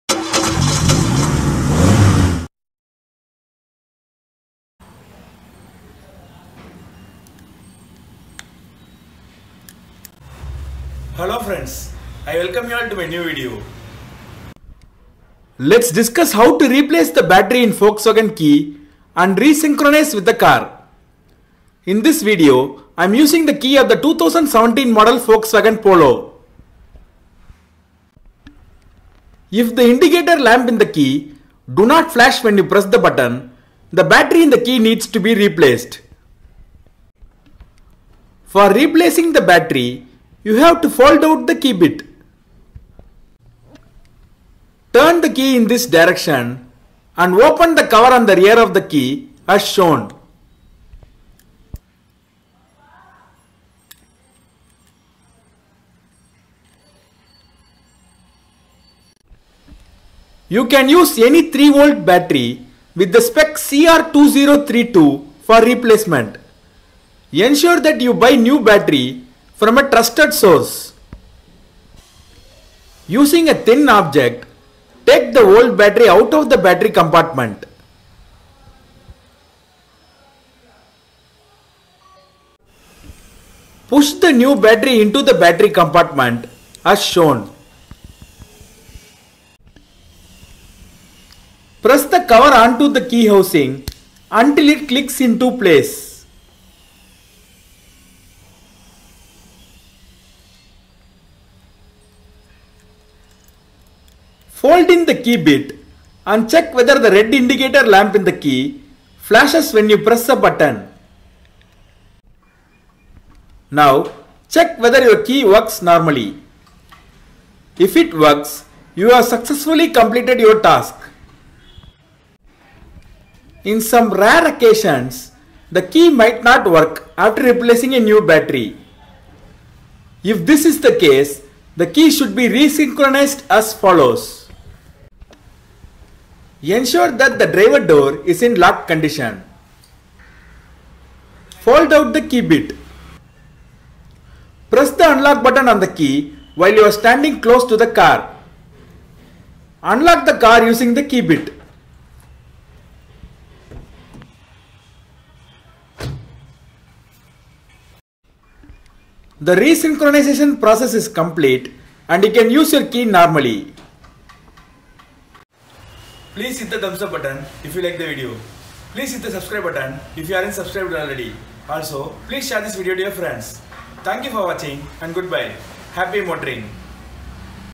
Volkswagen Key Battery Replacement & Re-Synchronisation : DIY
This Video Explains on How To Replace New Battery in a Volkswagen Key and How to Re-synchronise with The Car.

I am Using the Key of 2017 Model Volkswagen Polo in this video